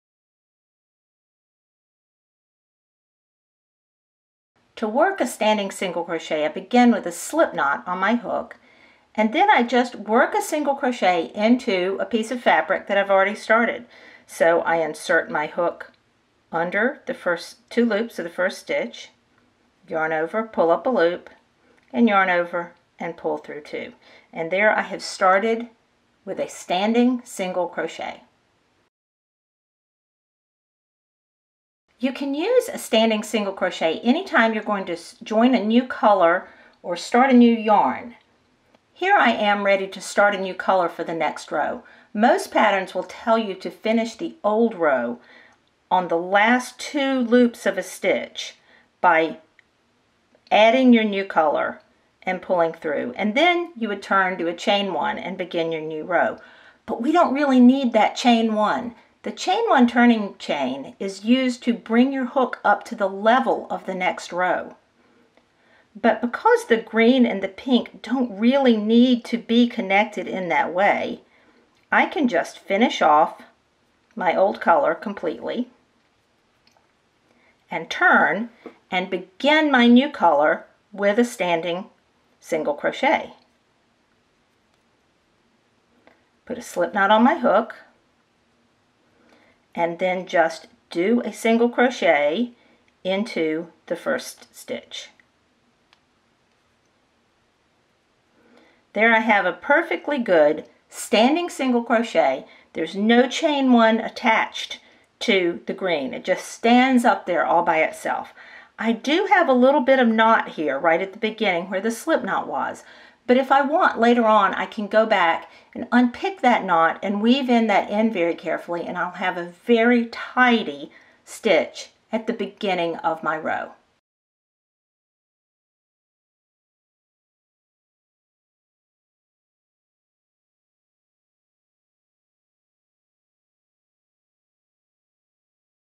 How to Work Standing Single Crochet
Standing single crochet is a wonderfully tidy way to start a new yarn or a new color. I named it that because it stands on its own, without relying on a turning chain. See how easy it is to do this simple technique, and how much it will improve the look of all your crochet!